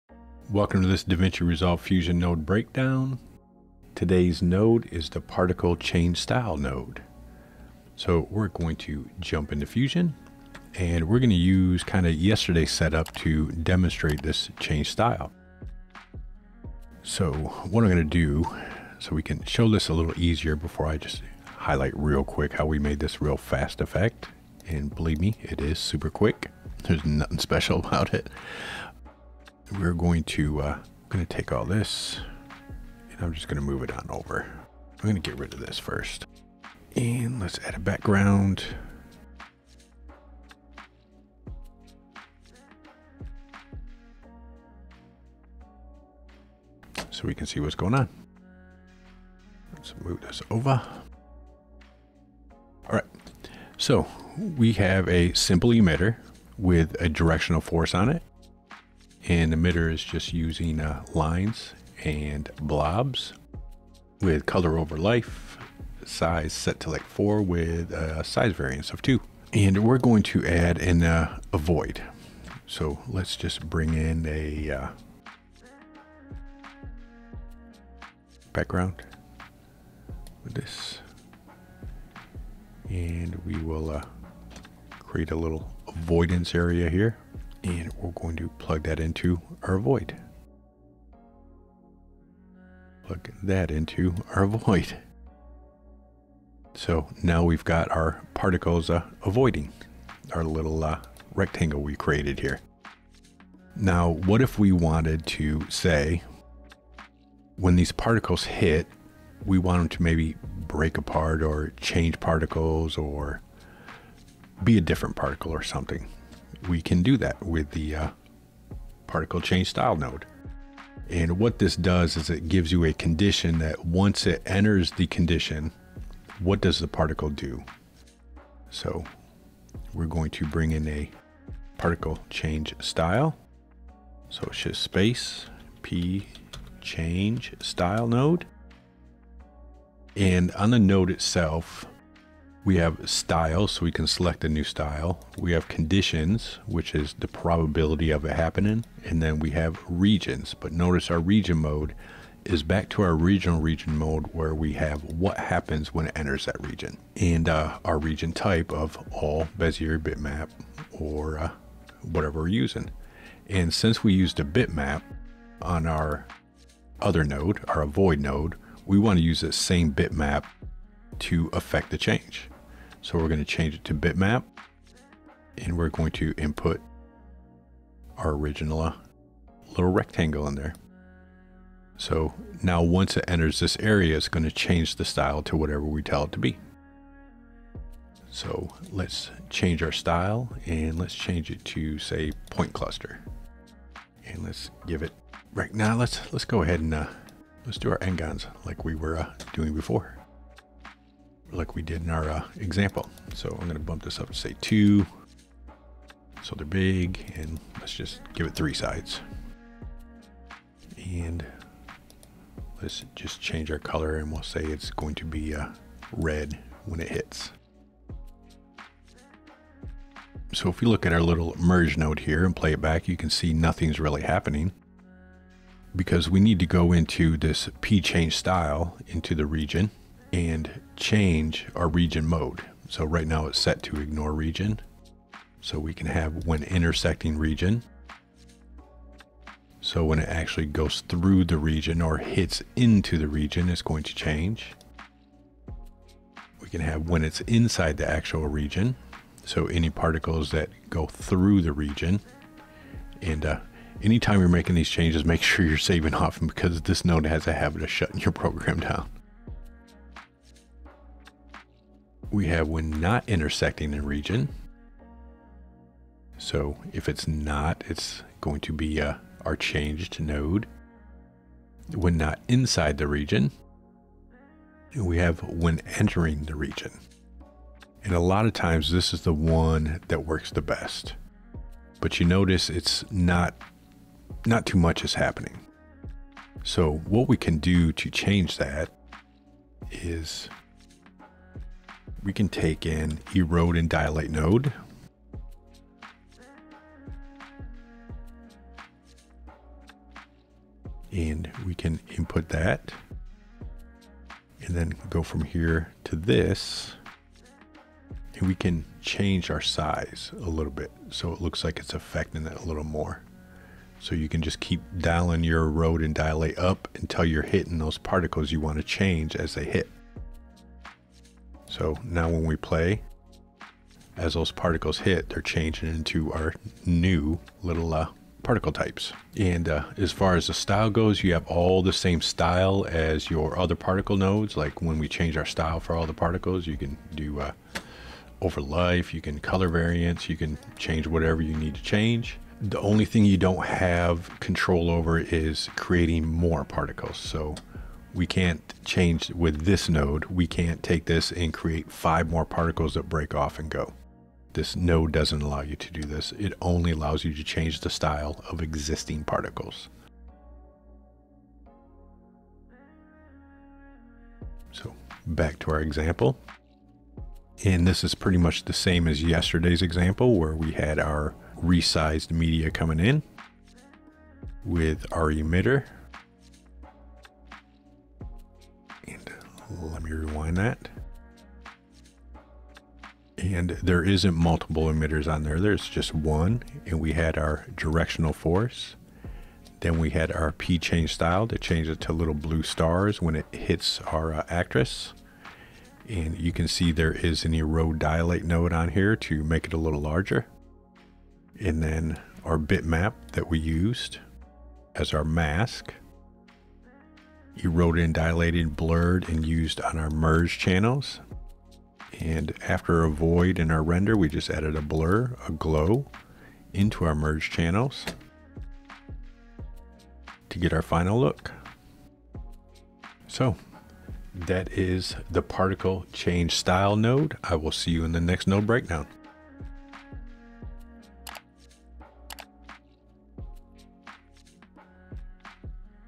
DaVinci Resolve Fusion Particle Change Style Node
Daily node breakdown of nodes used with DaVinci Resolve Fusion. In today's node we cover the Particle Change Style Node.